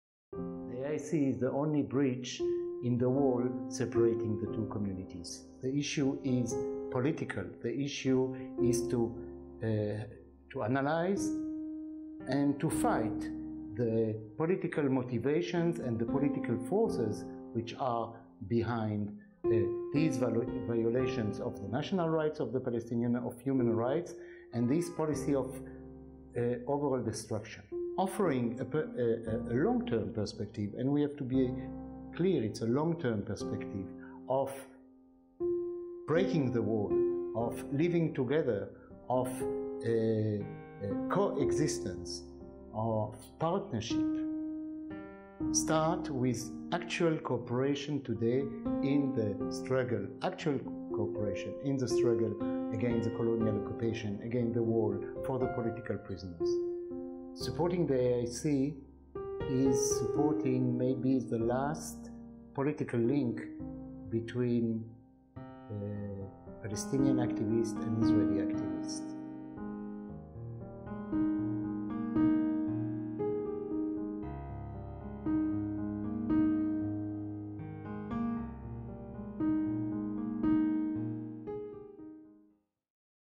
Now is the time for (co)resistance
The Palestinians, a people representing the aspirations and hopes of the entire world, are now tragically alone, enduring vicious attacks by the Israeli military and settlers.
We are launching an appeal now, not simply because of this current Israeli offensive, but as we know there are more to come and we must be prepared.
Inaction is a crime. For 30 years the Alternative Information Center (AIC) has brought together Palestinian, Israeli and international activists to resist apartheid and occupation.
Your support will allow the AIC to defy Israeli aggression. With your help, the AIC will initiate joint actions against settlers and settlements, while denouncing Israeli impunity.
We will concentrate our efforts in Jerusalem and Hebron, which today are at the centre of the conflict. We will demand an end to Israeli military and settler violence, disseminate information on Israeli impunity and organise activities to denounce Israeli colonial policies.
You can help make (co)resistance possible.